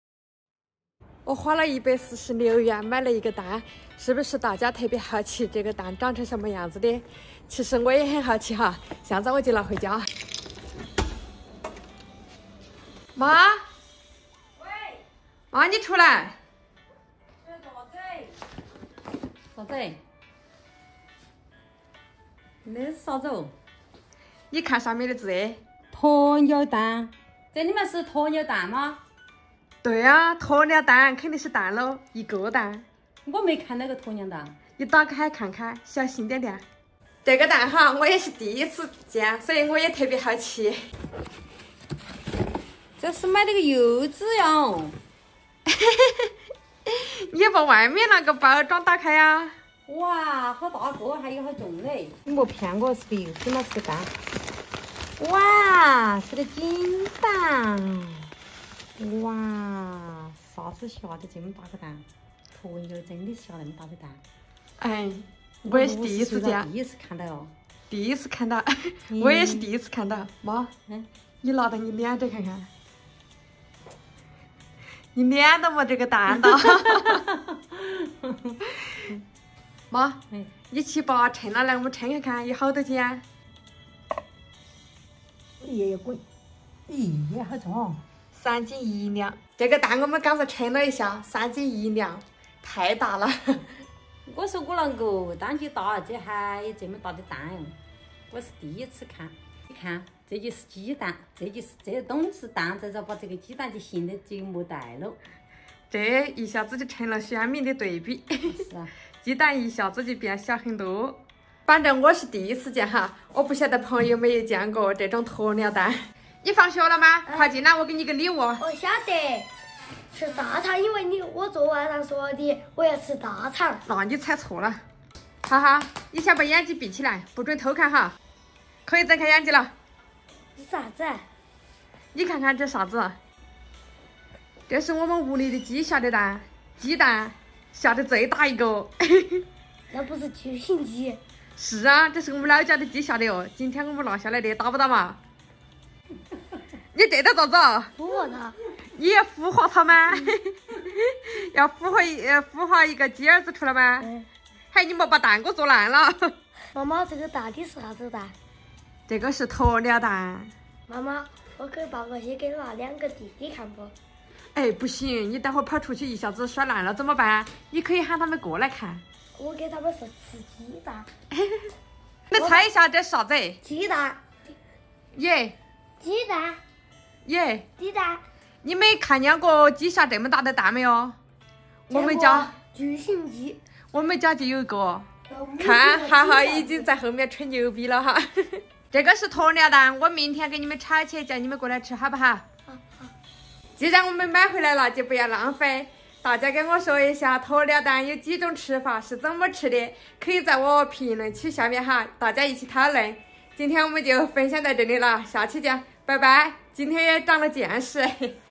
這兒媳真虎，花146元買1個蛋，一家人直呼真是第一次見，漲見識了【婷妹要努力】
婷妹花146元買了一個鴕鳥蛋，婆婆直呼第一次見到，豪豪還說要孵化它，真是可愛的童言童語。大家知道鴕鳥蛋要怎麼吃嗎？歡迎評論區討論！